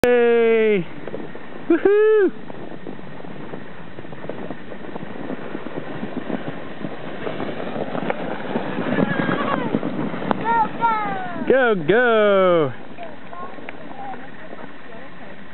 They're really flyin'!
Kayla and Brennan making a race out of it